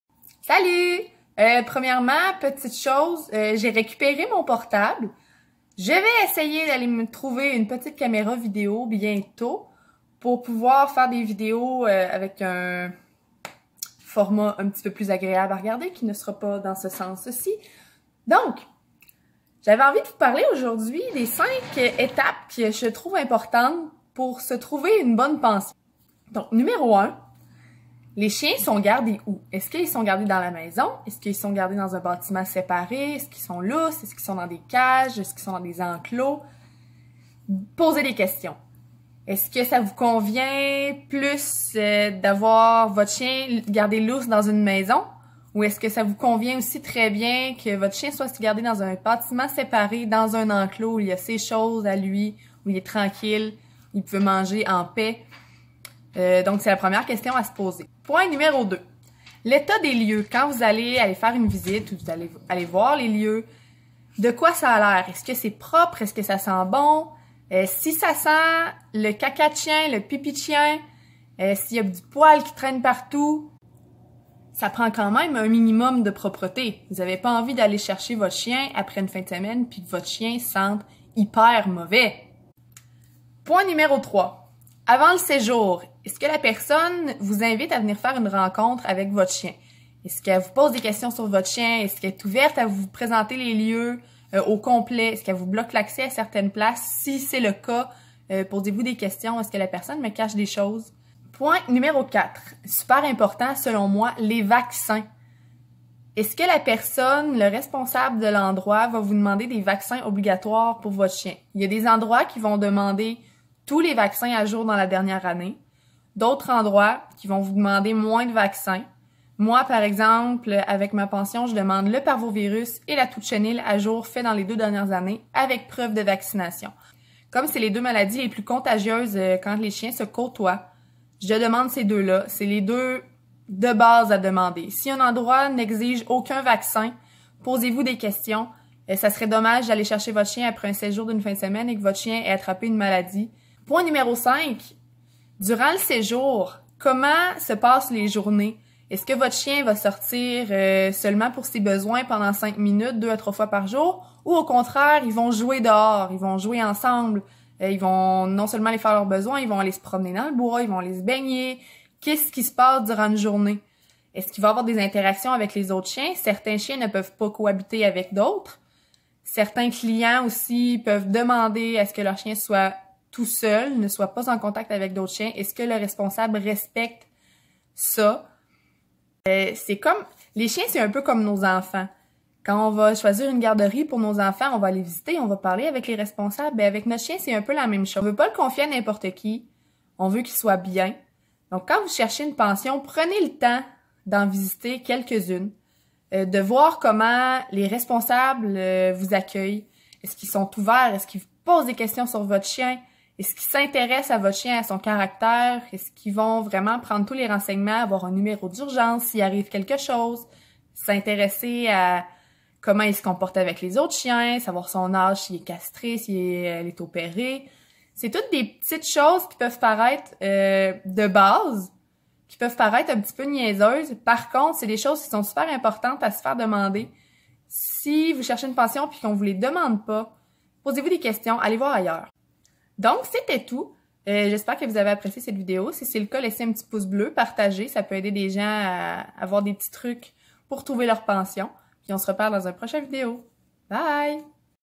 CHOISIR UNE PENSION CANINE EN 5 ÉTAPES
Les questions à ce poser avant de choisir une pension pour son chien.